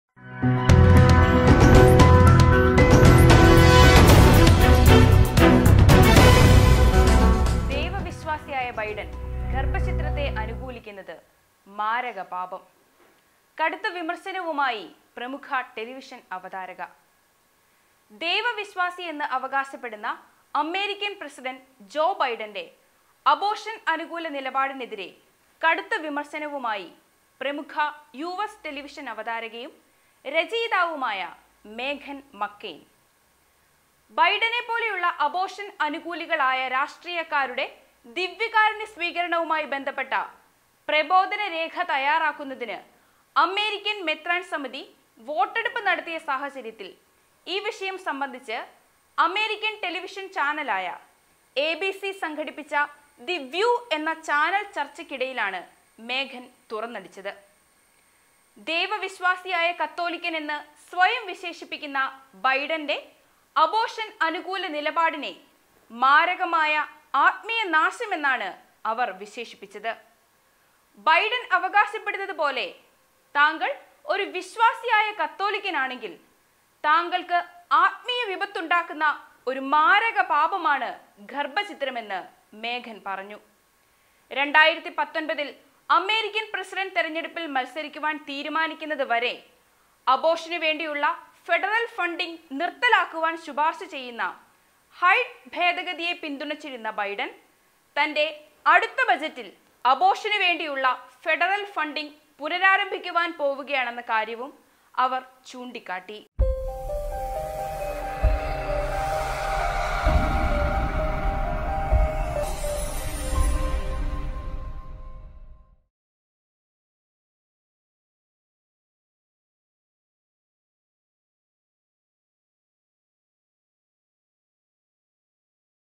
ബൈഡൻ ഗർഭഛിദ്രത്തെ അനുകൂലിക്കുന്നത് മാരക പാപം വിമർശനവുമായി അവതാരക
Topic - ബൈഡൻ ഗർഭഛിദ്രത്തെ അനുകൂലിക്കുന്നത് മാരക പാപം വിമർശനവുമായി അവതാരക
Directed and Produced By Mr. Thomas Kurian/ Bethlehem TV